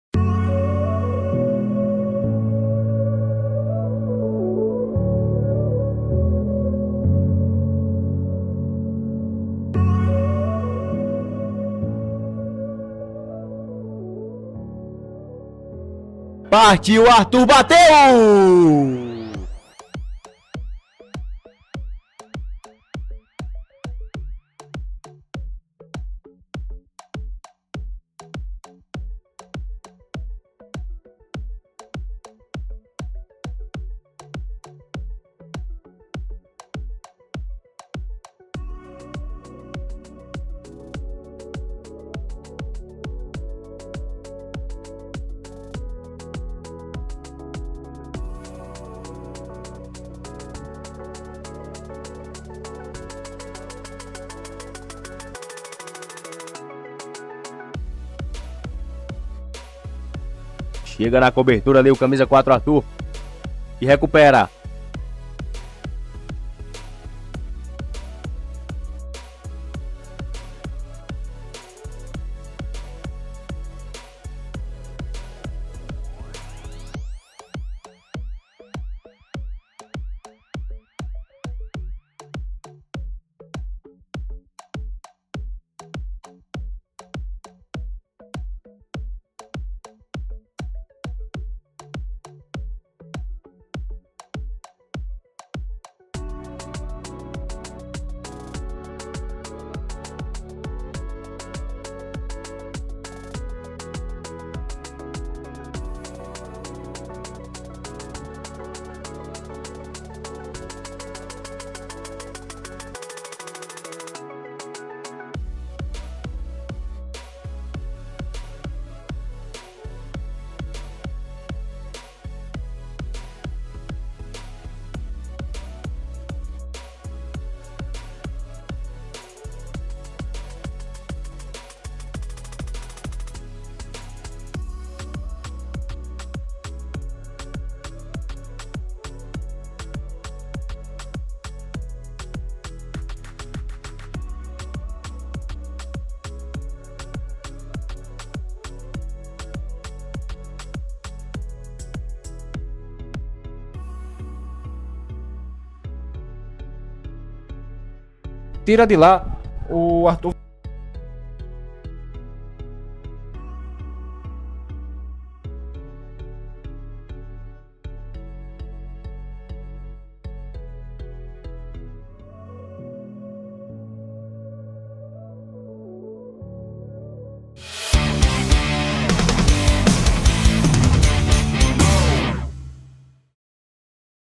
Arthur - Zagueiro/Defensor - 2024
Seleção de melhores lances do zagueiro Arthur, em atuação pelo CRB na temporada 2024.

Edição: Bruno Michel
Futpress, 2024.